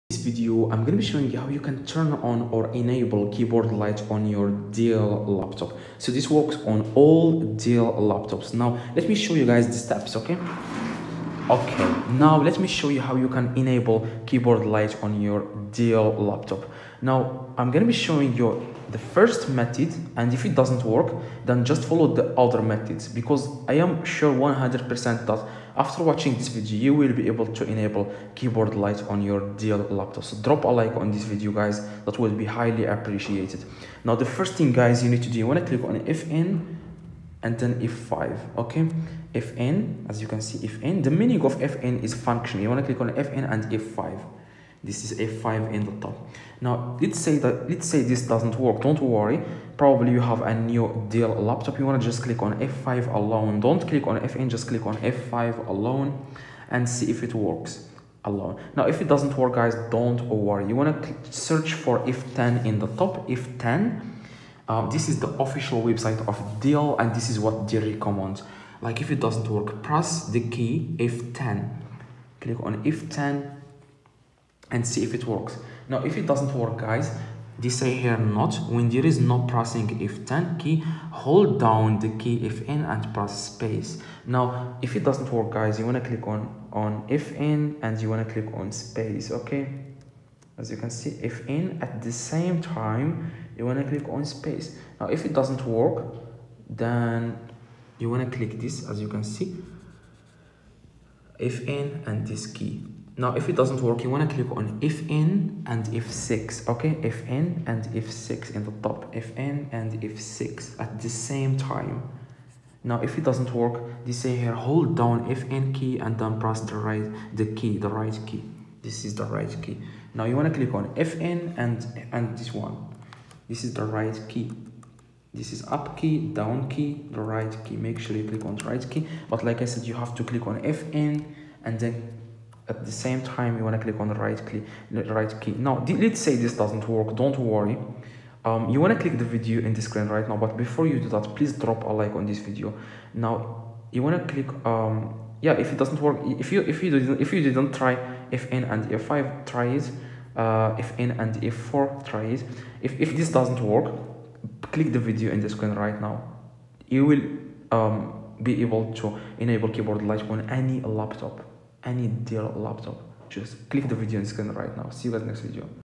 How to Turn On Keyboard Backlight On Dell Laptop ! (Enable Keyboard light)
DO YOU want to enable backlit on your dell laptop? this vid is for you